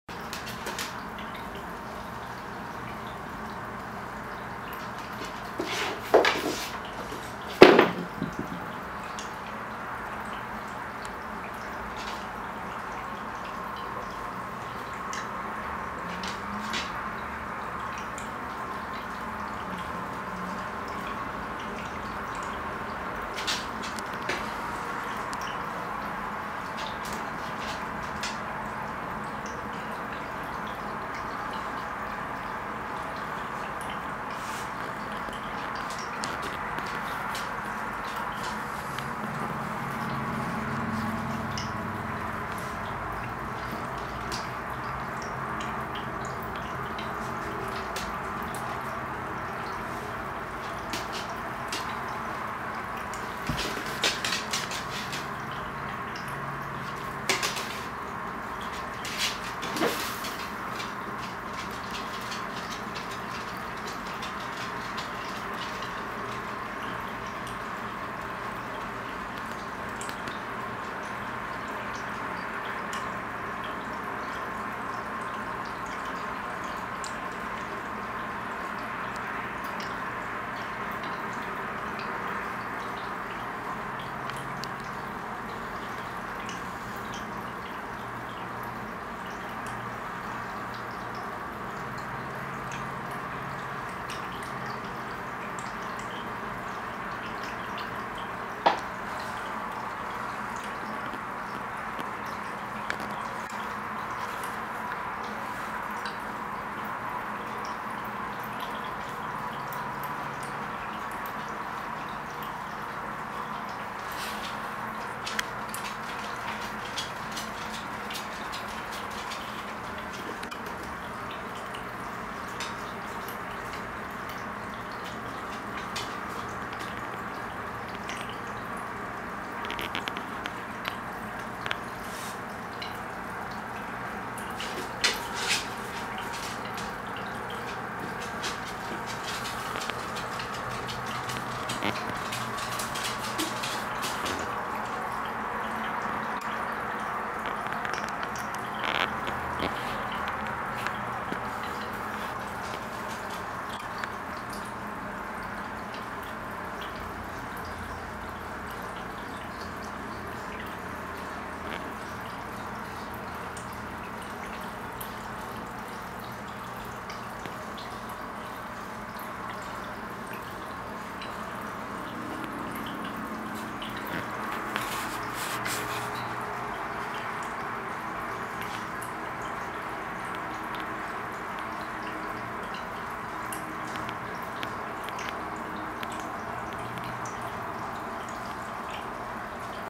Weather loaches (Golden Dojo loaches) in 20 gallon long tank
Two golden dojo loaches and one regular type (Misgurnus aguillicaudatus) and a fantail goldfish (um... Goldfishius fantailius).  I've had them for several months now.  The loaches are most active in the morning and evening.  This was about 5 PM, when I fed them.